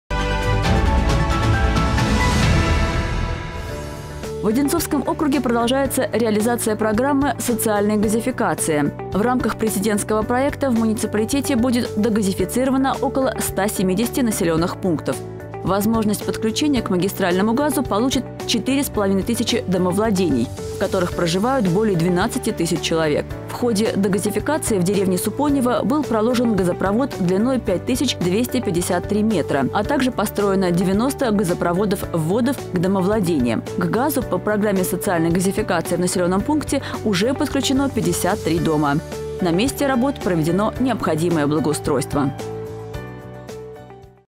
Догазификация деревни Супонево
В Одинцовском округе продолжается реализация программы социальной газификации. В ходе догазицикации в деревне Супонево проложили газопровод длиной более 5 километров.